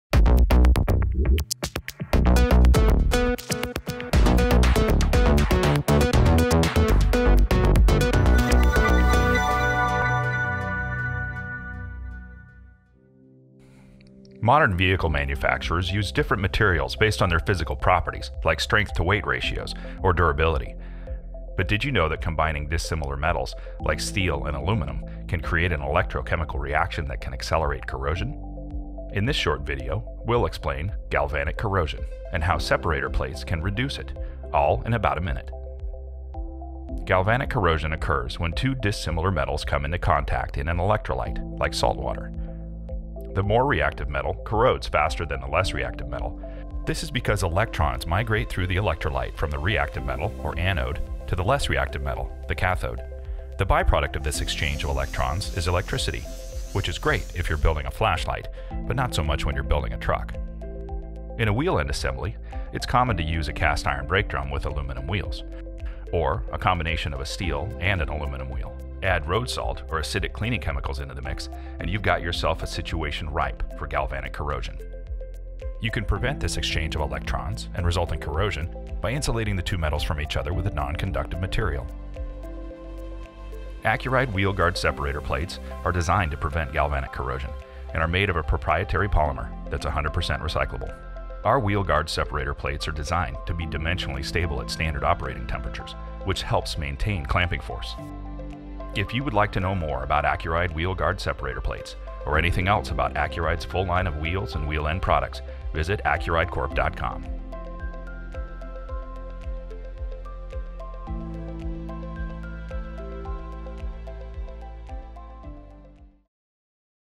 Tech Minute: Galvanic Corrosion and Wheel-Guard®
De-icing compounds and road salt can have corrosive effects on vehicle components, but did you know that combining dissimilar metals can accelerate this corrosion? In this tech minute: galvanic corrosion.